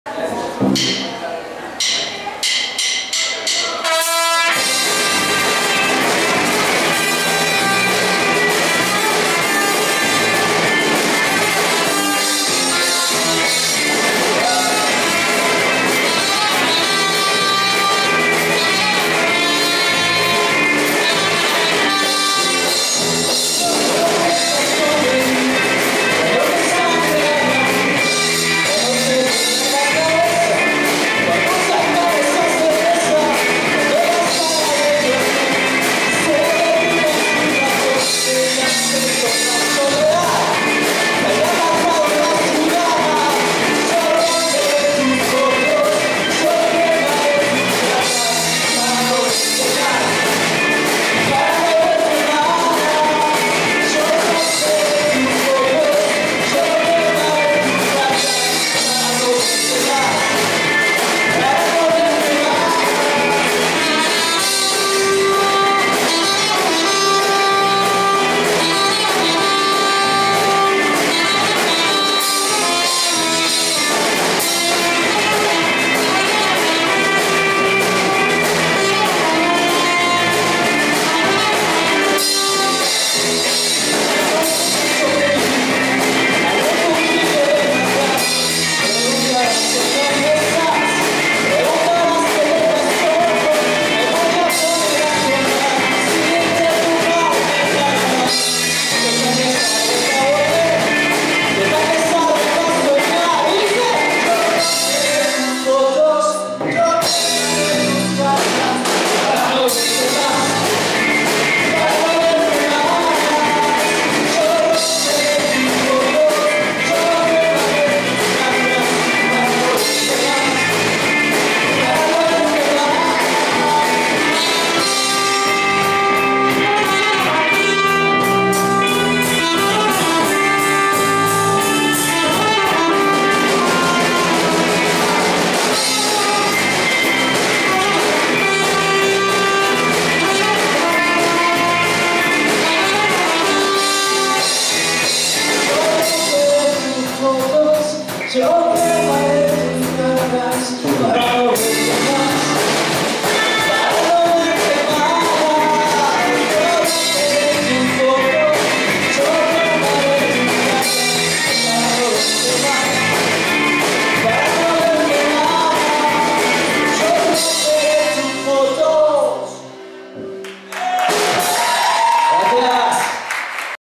4 - Obsidiana
Obsidiana en el Battleground Rock-Fest 2018 por Ciudad del Rock Eterno.
interpretando el tema:

10 de Marzo del 2018